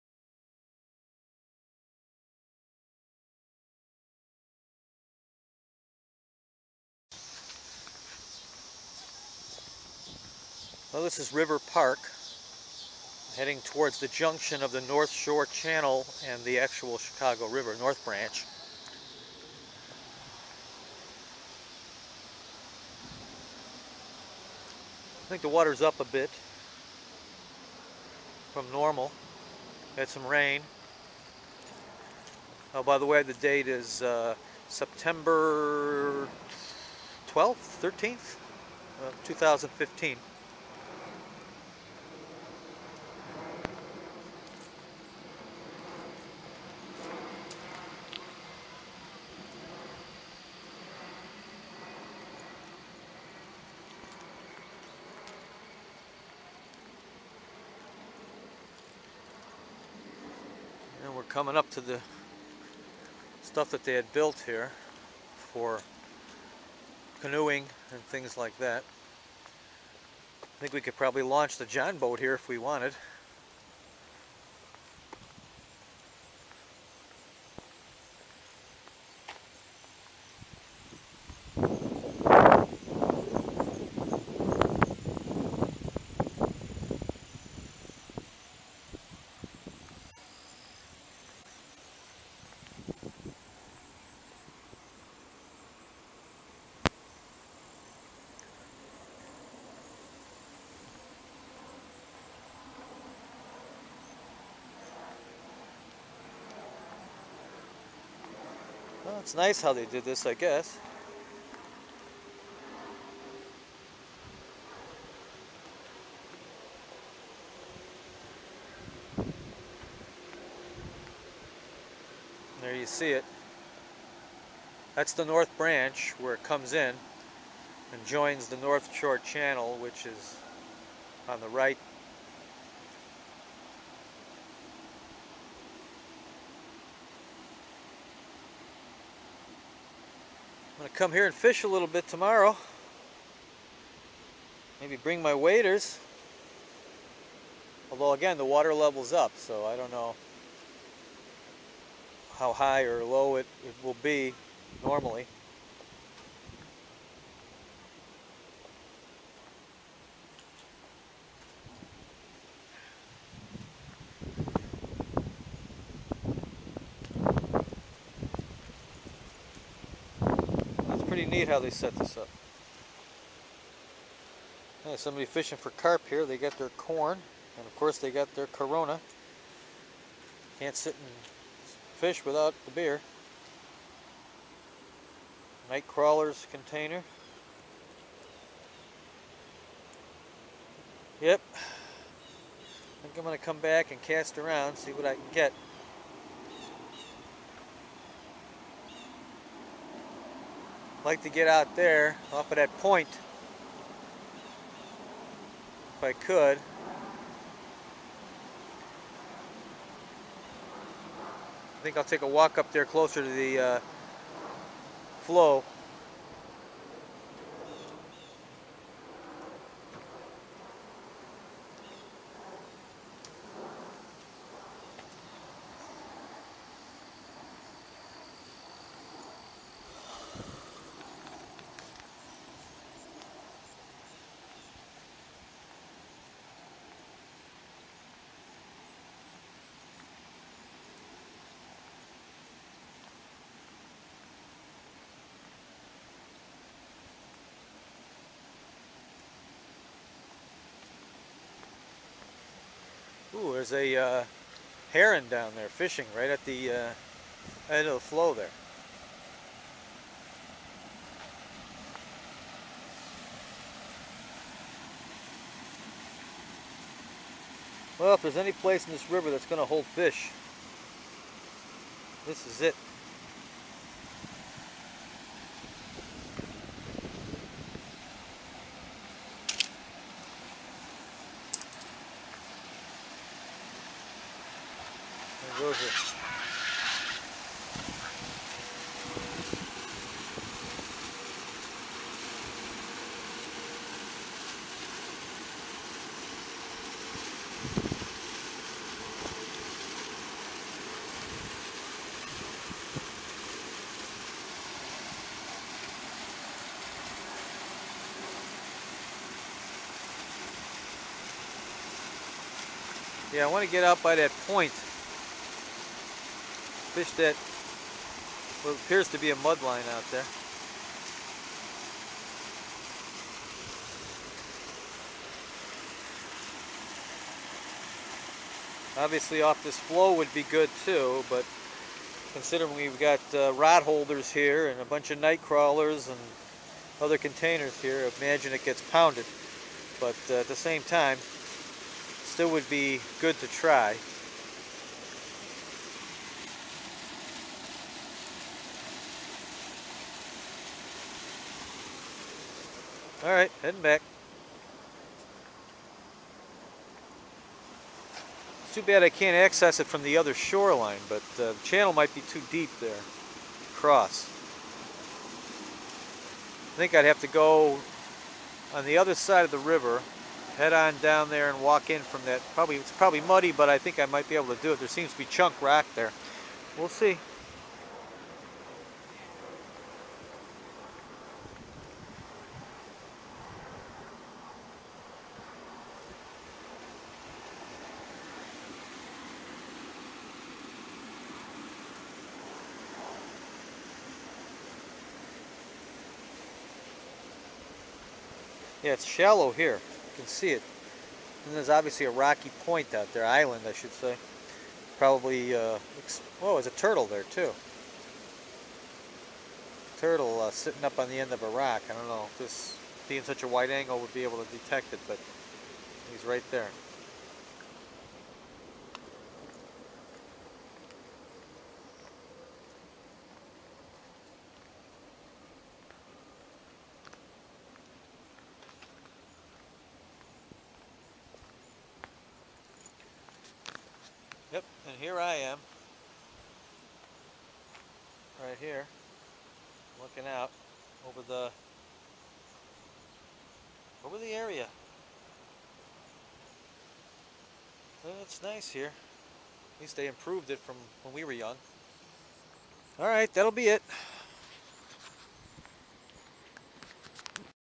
Chicago River and North Shore Channel Junction @ River Park
This place used to be a shabby excuse for a fishing spot inhabited by drunks, hooligans and other assorted losers but now the Chicago Park District has fixed it up real nice. The addition of a canoe launch is an awesome addition.